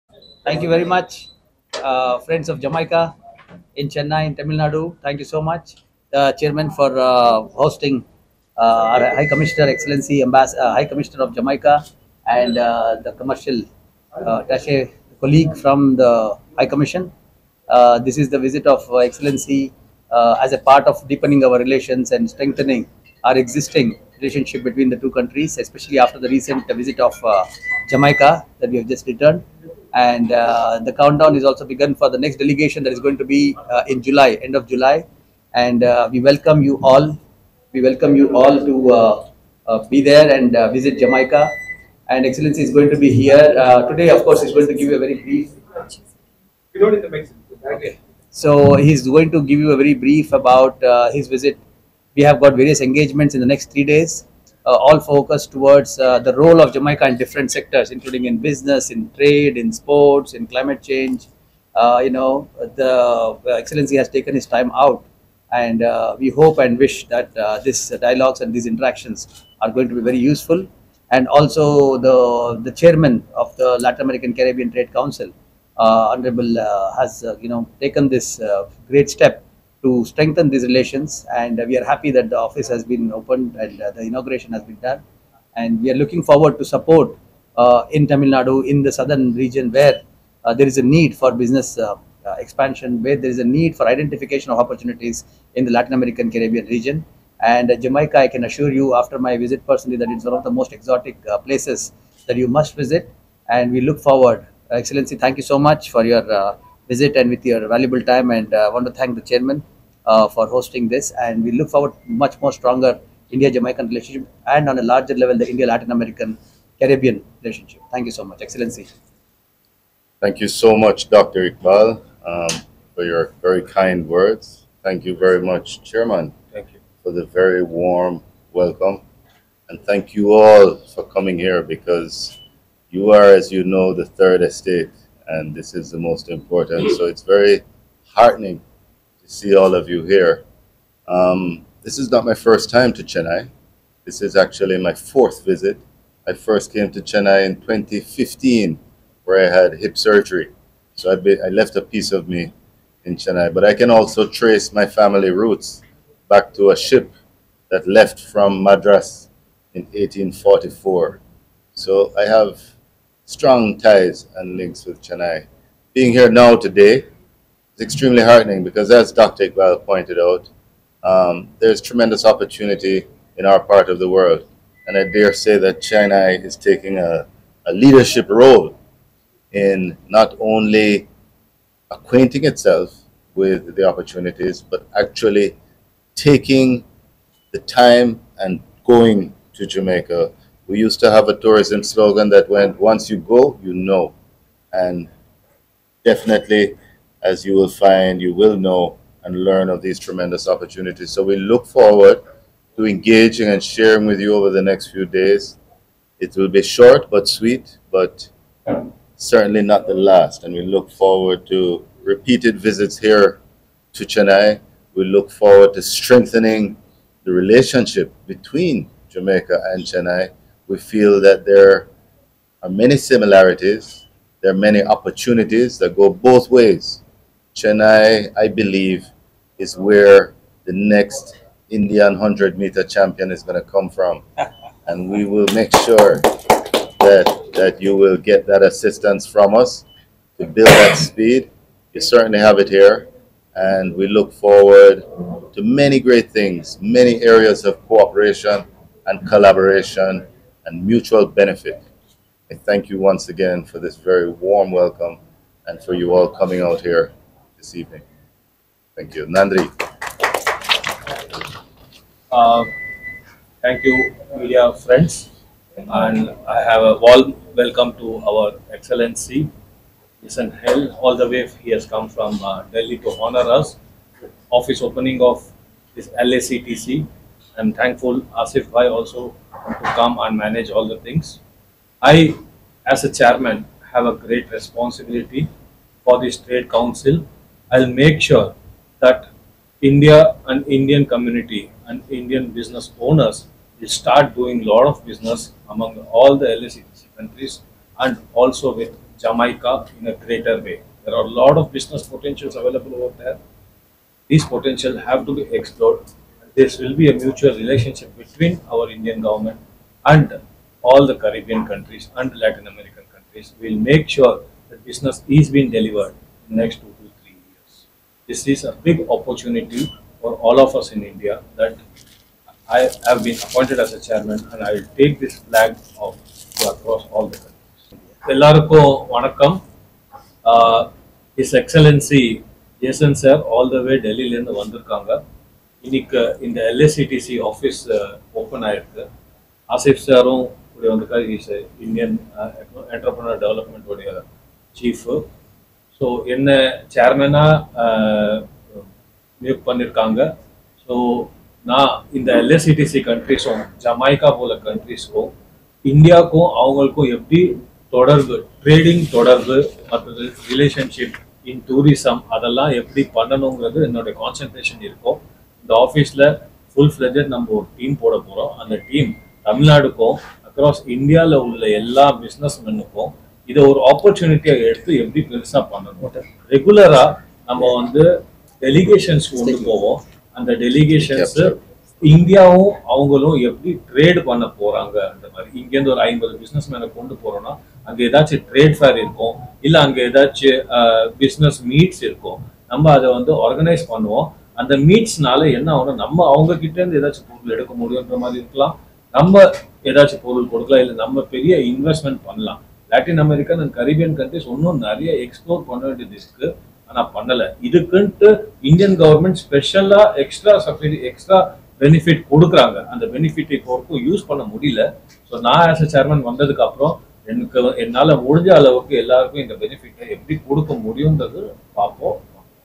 Latin American Caribbean Trade Office inaugurated in Chennai
The LATIN AMERICAN CARIBBEAN TRADE OFFICE(LACTC) in association with the Indian Economic Trade Organization inaugurated its office in Chennai which was well attended by the Business community in Tamil Nādu state and other parts of India.  The Ambassador of the Republic of Jamaica His Excellency Jason Hall and the newly Installed Chairman of the LACTC Hon. Rajesh Chandan inaugurated the office in Chennai headed by the dynamic team of specialists in international business sectors.  The welcome keynote address was given by the Chairman of LACTC who also gave a presentation of opportunities available for foreign investors in the education sector and other collaborative projects with the state.

Dr. Asif Iqbal, President of the Indian Economic Trade Organization who just returned from the successful delegation from Jamaica recently briefed the community about the visit to the beautiful country of Jamaica a few weeks ago with a 9 member delegation also mentioned the opportunities available for the Indian businessmen in areas of Tourism, IT Development, solar energy, BPO, hospitality sector and also education collaborations.

The Latin American Caribbean Trade office is responsible for Promoting Investment and Growth in collaboration with the Ministries of trade and commerce of various latin American countries and their governments and trade promotion departments. The office in Chennai will be under the embassies of various Latin American Caribbean embassies in New Delhi.  The event was attended by around 120 participants representing public, business, educational and healthcare sectors of the country.